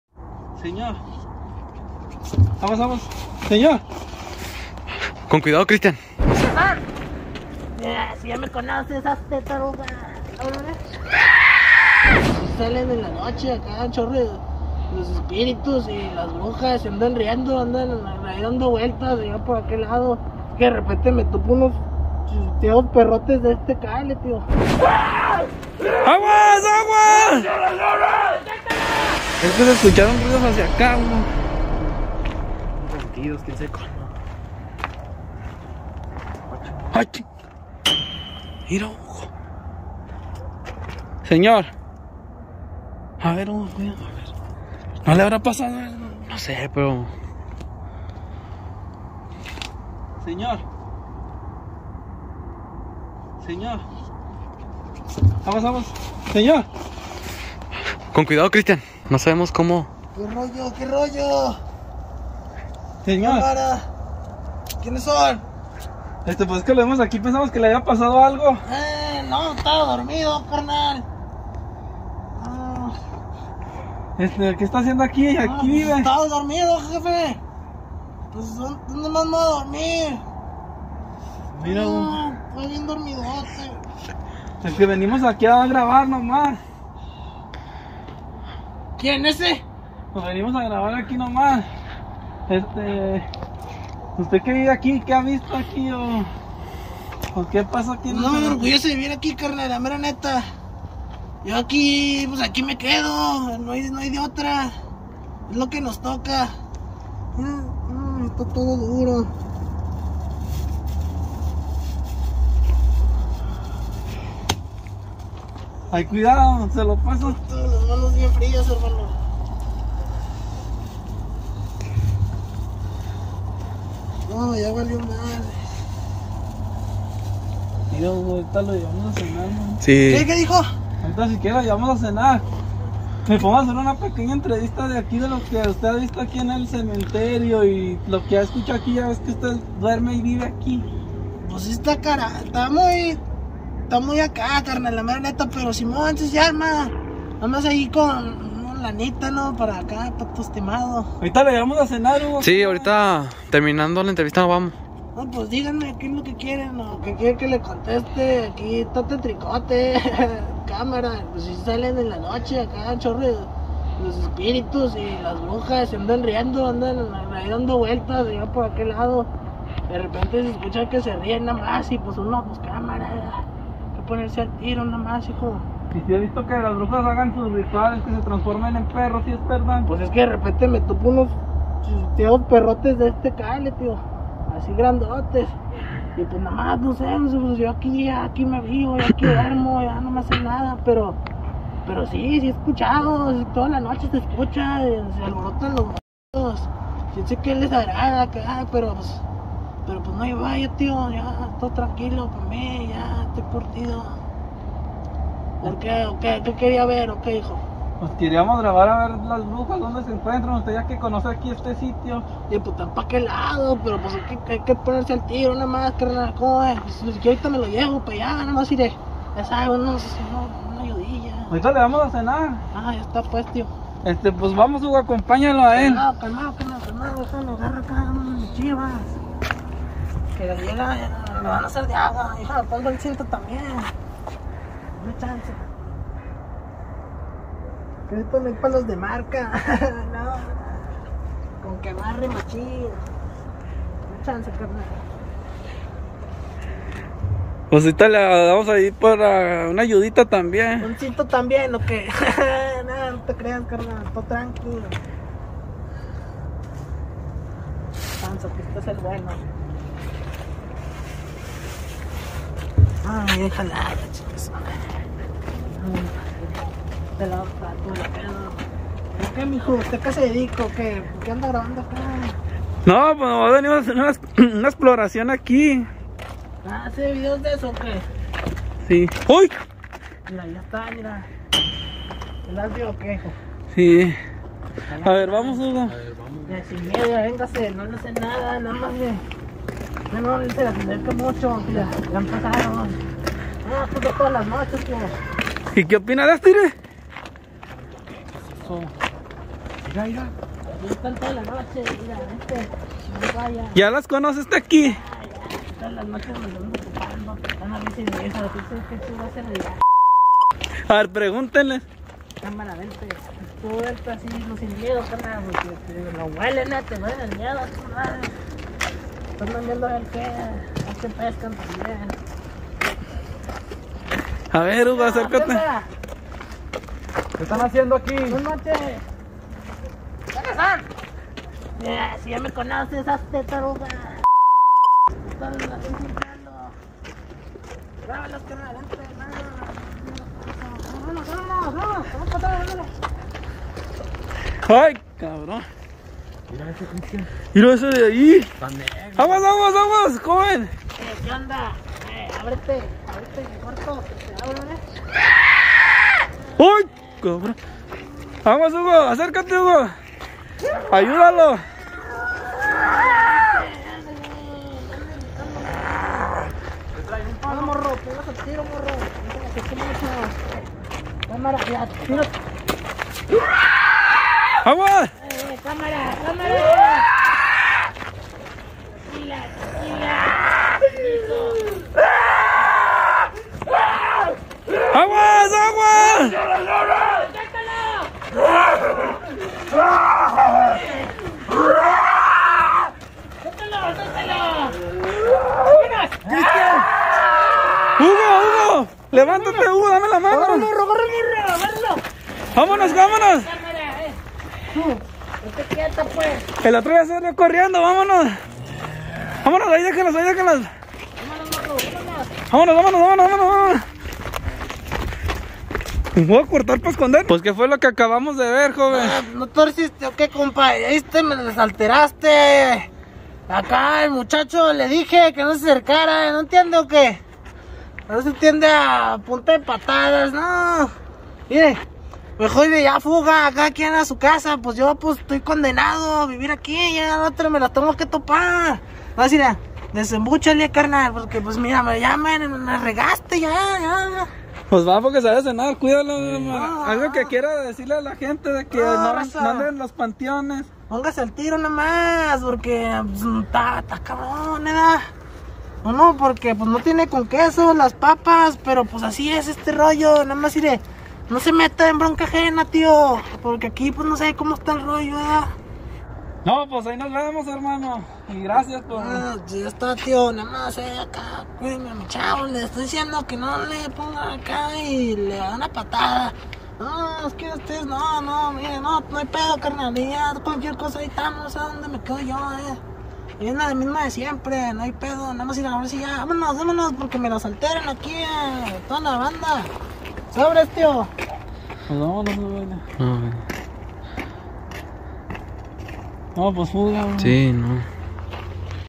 EL HOMBRE QUE DUERME EN | tumbas y vive con brujas
Nos encontramos a una persona en un cementerio y esto fue lo que nos conto